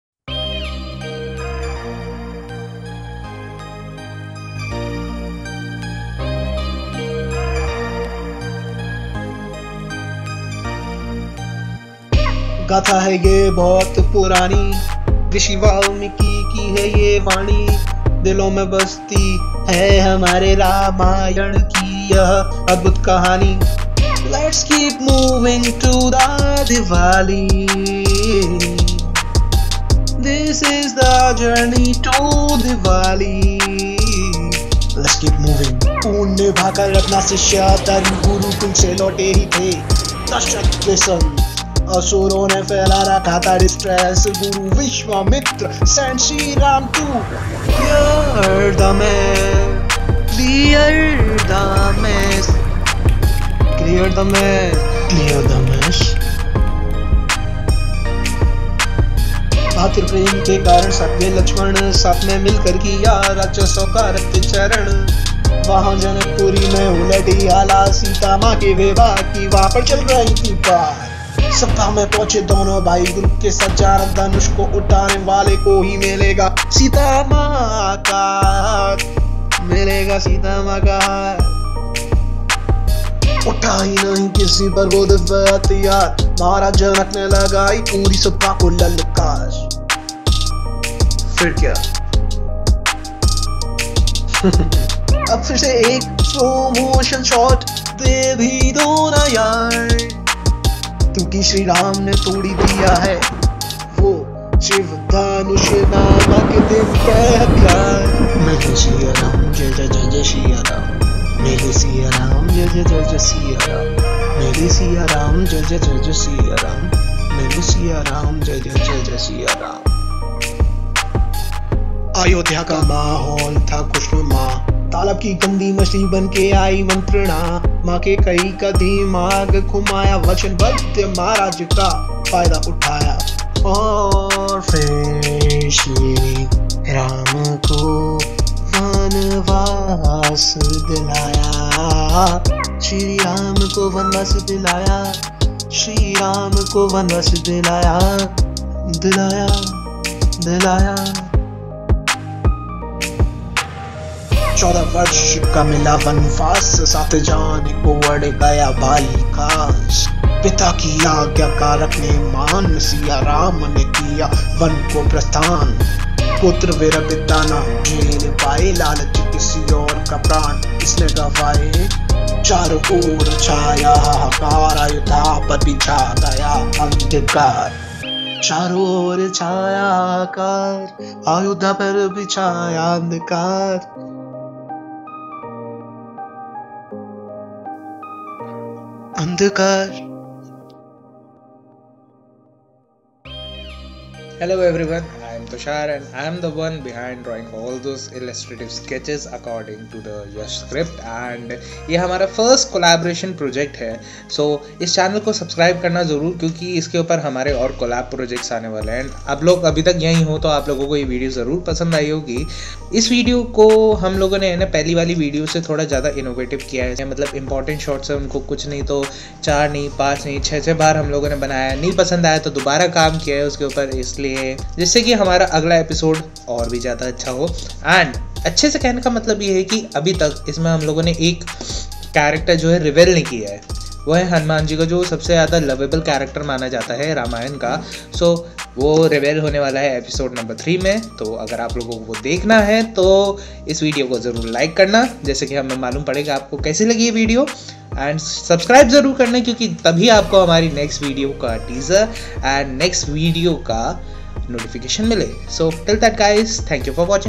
JOURNEY TO DIWALI | E2 |RAYAMANA ANIMATED MANGA | A WEB SERIES BY YASH BHARADWAJ ft. TUSHAR BANSAL
This is a new series for celebrating  Diwali where we are making these Indian manga-style rap videos for you.
This is the second episode of the series  JOURNEY TO DIWALI where our heroes  are up for the journey  to kill demons and get the queen but isn't  it  to direct there is a twist at the end so please watch the complete episode.

Timestamps:
00:00 - Intro
00:30 - Episode 2: it's  all Mess
03:12 - Outro